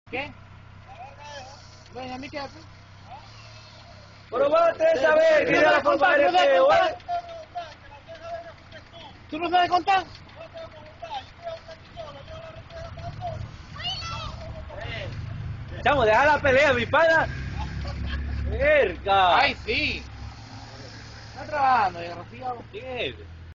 POLICIAS VENEZOLANOS CONTRA CIVILES (CONFLICTO)
Policía Municipal de Maturin 23 ago